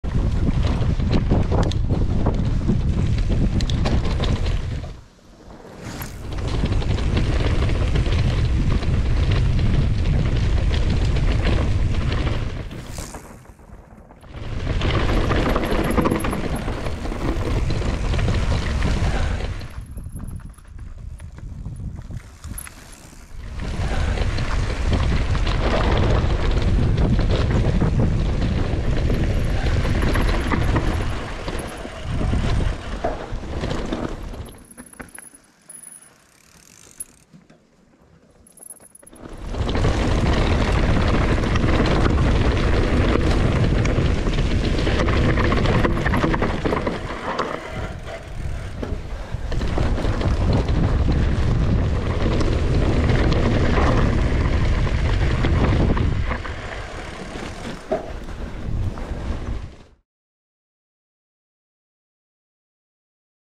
MTB White Peak Descent to Chee Dale - Mountain Biking Peak District
Known on Strava as "Down in tut dale", this is a great little descent with several switchbacks on the way down. Just a short video, trying some different techniques, including off the bike. I just do it for fun, so I know my technique may leave a little to be desired! Hot and sunny weekday morning with the hills to myself.